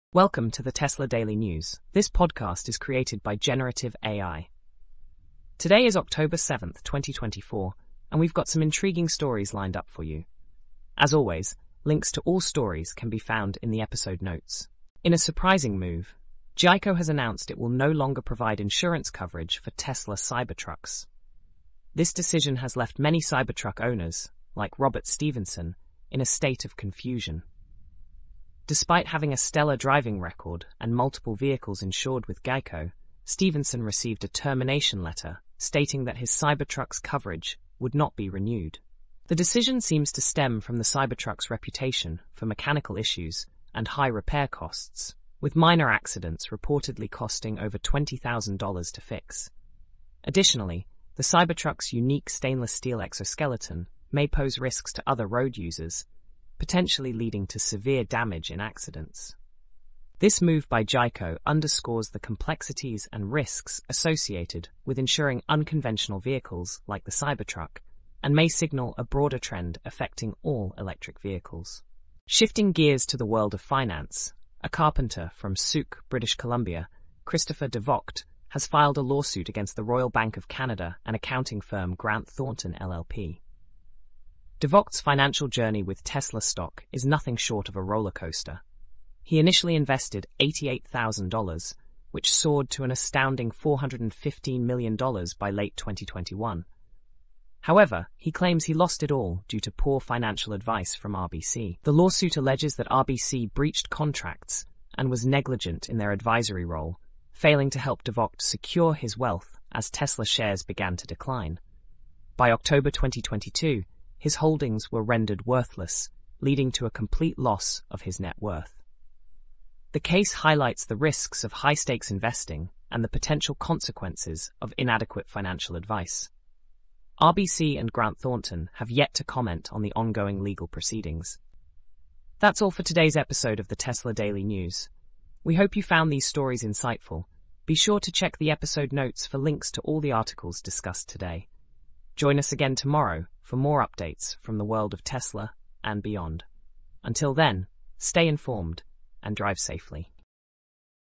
Tesla News for Oct 07, 2024
-Boost your business with Ramp! Save time and money with smarter expense management. $250 credit

Today's topics:
-GEICO drops Cybertruck coverage
-Cybertruck insurance challenges
-Tesla stock investment lawsuit
-Financial advice and investment risks